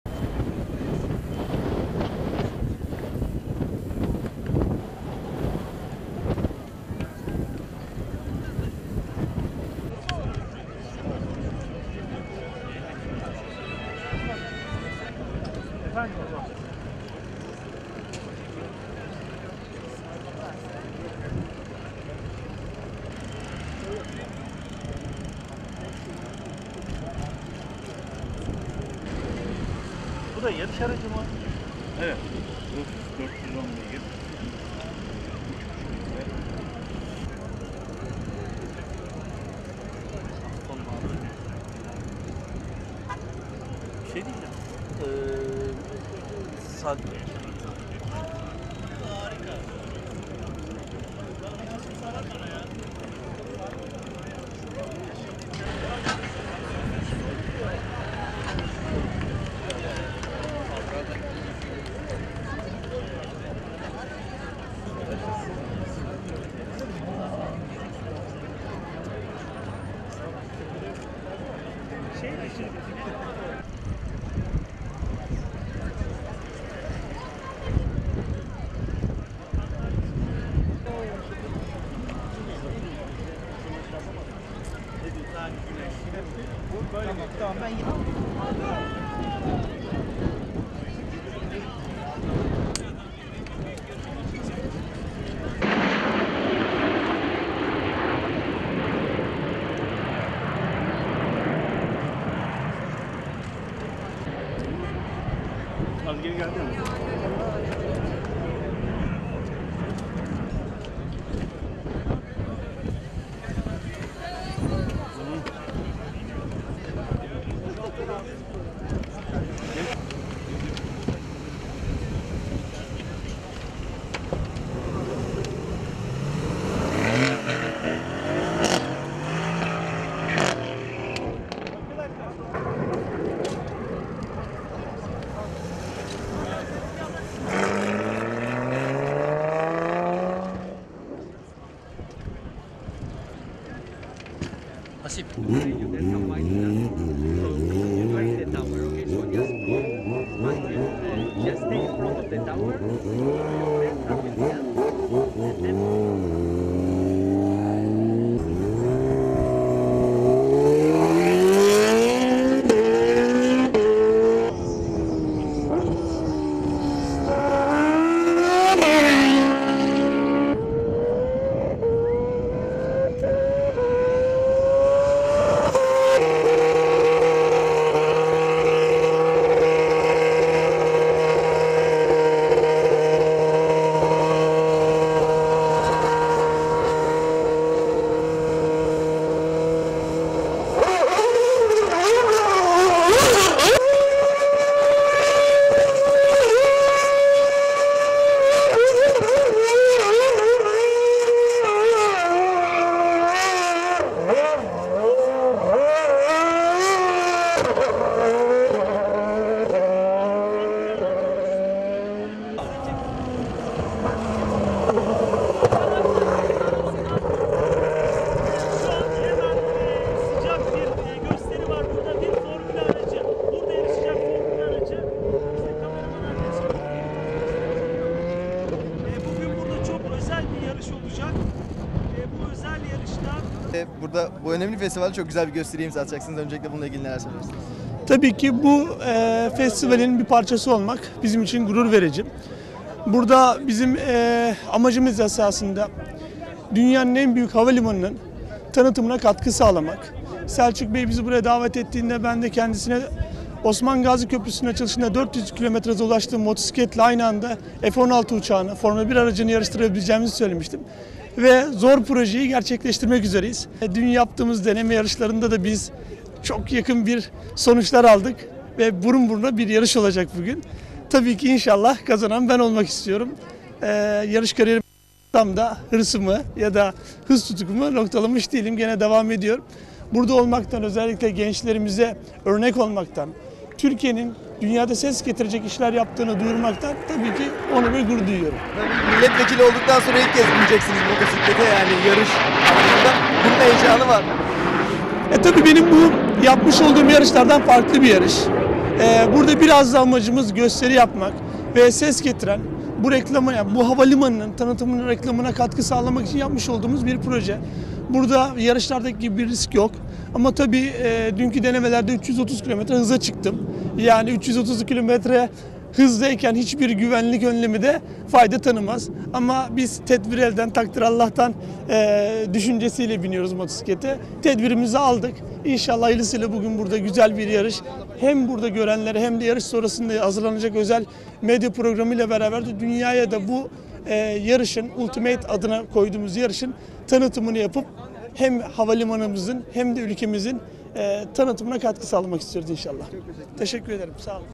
Teknofest'te Kenan Sofuoğlu rüzgarı
Teknofest İstanbul Havacılık, Uzay ve Teknoloji Festivali'nde dev yarış gerçekleşti. Festival kapsamında düzenlenen ve bir adet formula aracı, Solotürk F-16 uçağı, bir iş jeti, bir adet Kawasaki model motosiklet ile spor araçların katıldığı yarışı Kenan Sofuoğlu motosikletiyle 9,43'lük derecesiyle kazandı.